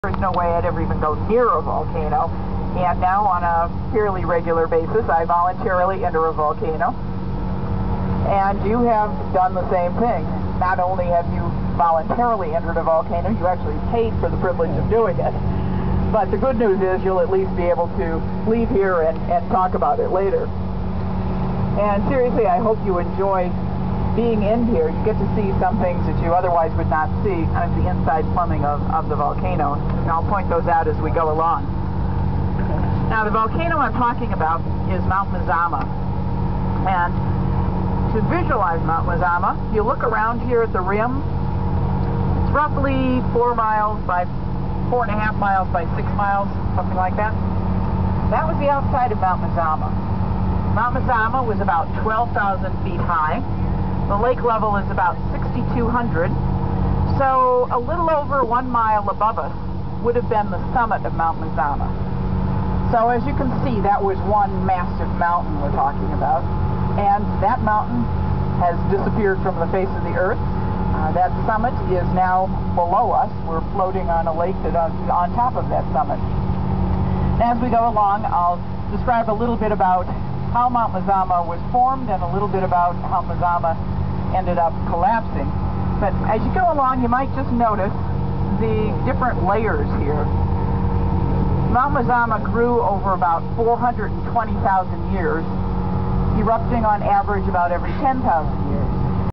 Crater Lake - a little history
History provided by our tour boat guide on Crater Lake, OR.